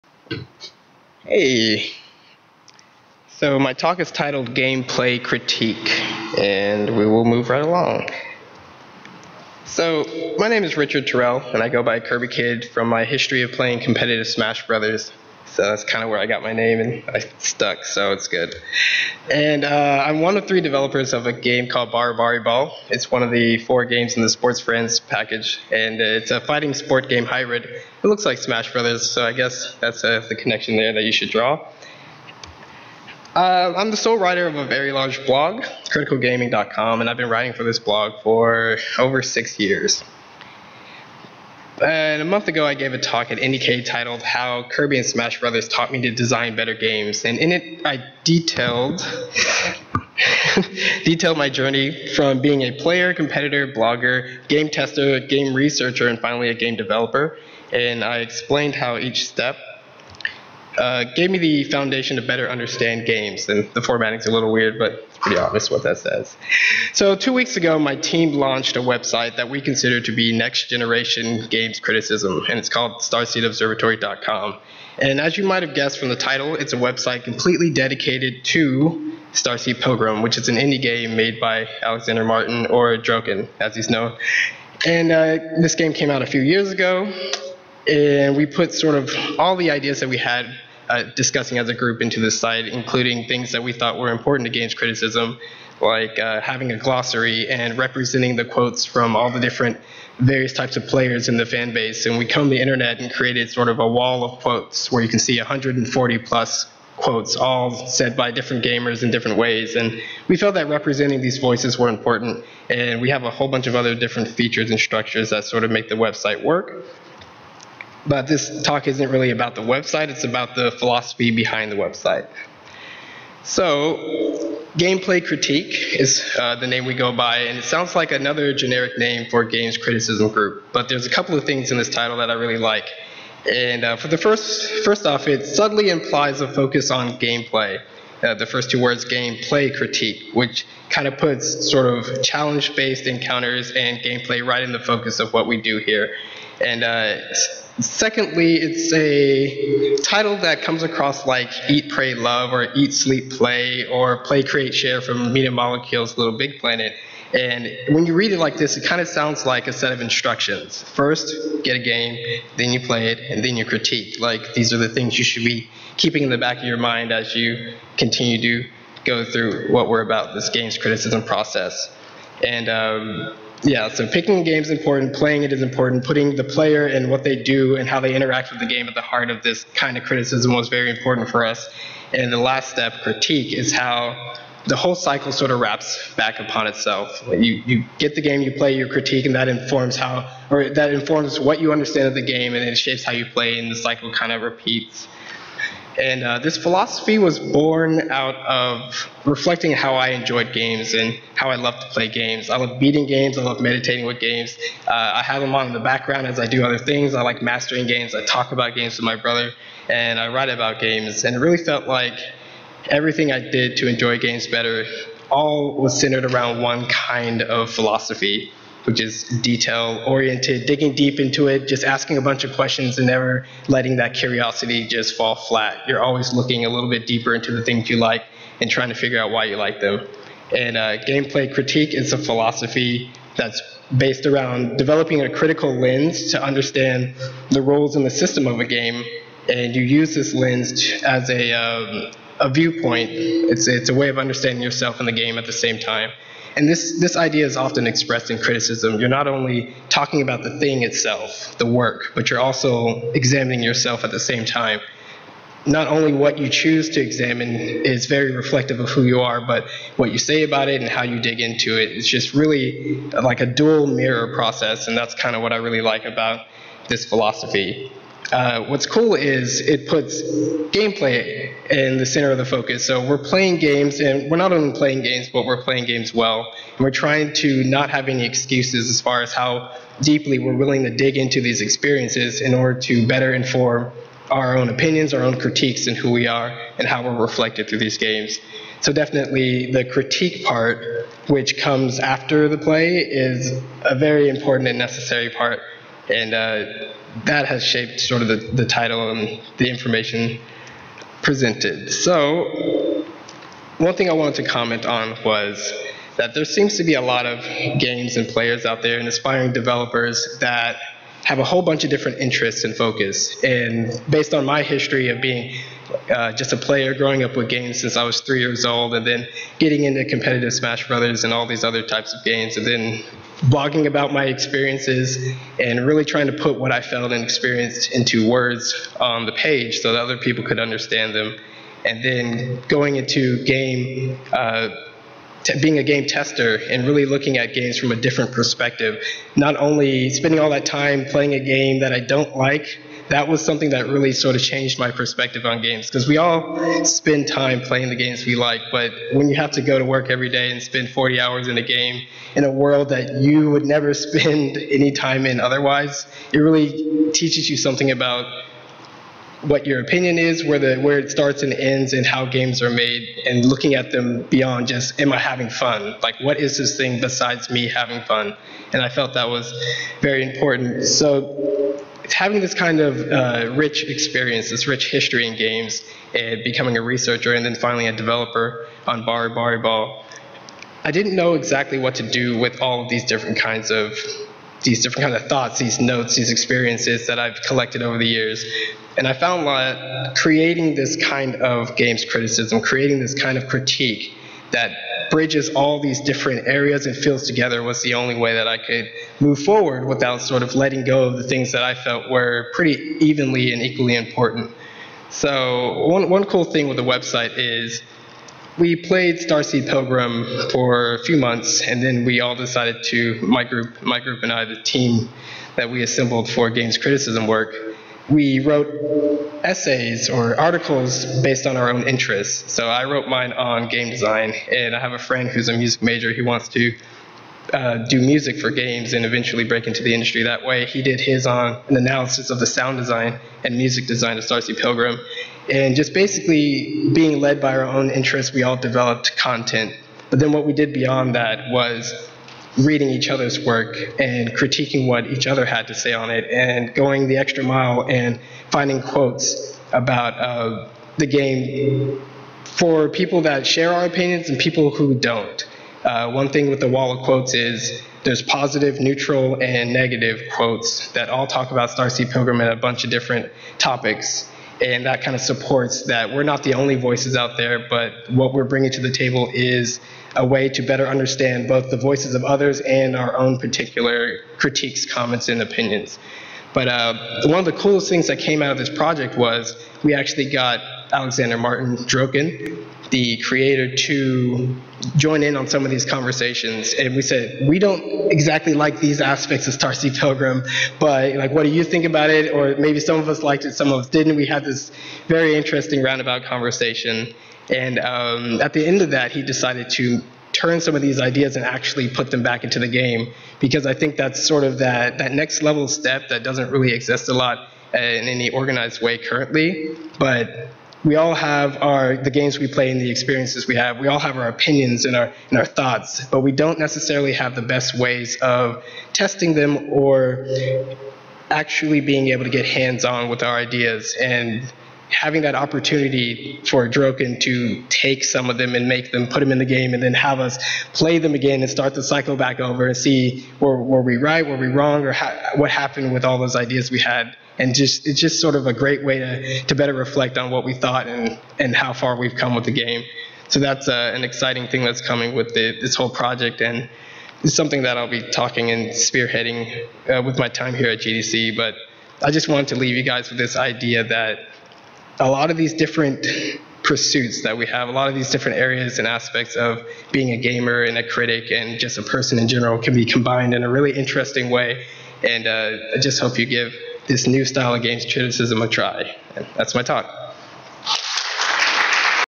Game.Play.Critique - Richard Terrell - Outward
Richard Terrell discusses his method and reasons for bringing players, critics, and developers together.